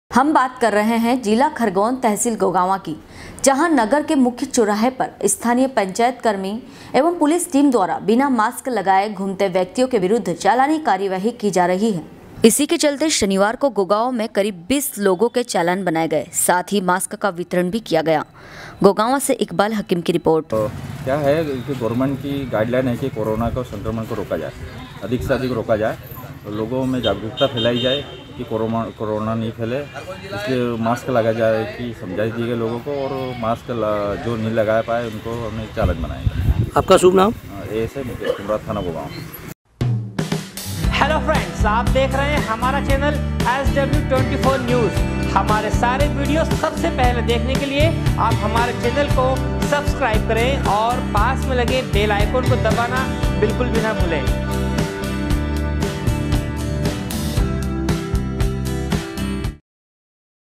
SW 24 News Madhyapradesh
हम बात कर रहे है जिला खरगोन तहसील गोगावां की जहां नगर के मुख्य चौराहे पर स्थानीय पंचायत कर्मी एवं पुलिस टीम द्वारा बिना मास्क लगाएं घूमते व्यक्तियो के विरुद्ध चालनी कार्यवाही की जा रही है । गोगावां से इक़बाल हकीम की रिपोर्ट